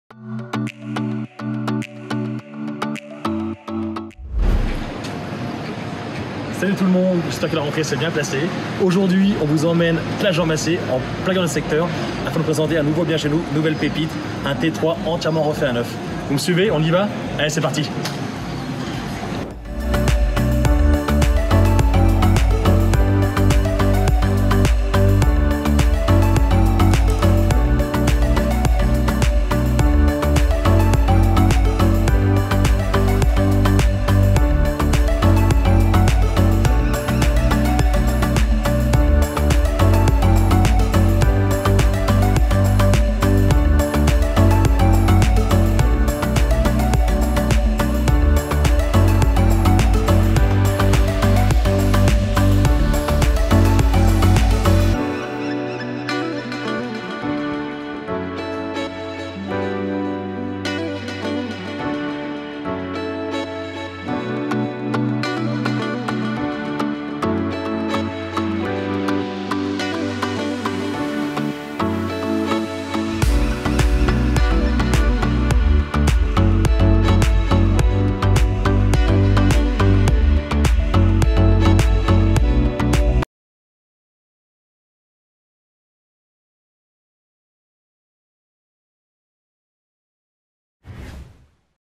Appartement Jean macé type 3 70.27 m2
RARE A LA VENTE !
C'est à quelques mètres de l'une des places les plus prisées de Lyon, la place Jean Macé que nous vous proposons notre nouvelle exclusivité. Située dans un quartier très branché où vous trouverez toutes les commodités, commerces de proximités, restaurants, cinéma, écoles, transports, métro, tramway, bus, gare et les quais du Rhône ainsi que le parc Blandan à quelques pas.
Au premier étage de ce bel immeuble ancien vous découvrirez un appartement de type 3 d'environ 70,27 m2 composé d'un séjour lumineux, une cuisine meublée et équipée semi-ouverte sur le séjour, un dégagement qui dessert une salle de bains avec WC séparé, et deux vastes chambres.
L'appartement totalement rénové marie subtilement l'ancien et le récent.
A visiter sans tarder. Possibilité de louer un garage à proximité.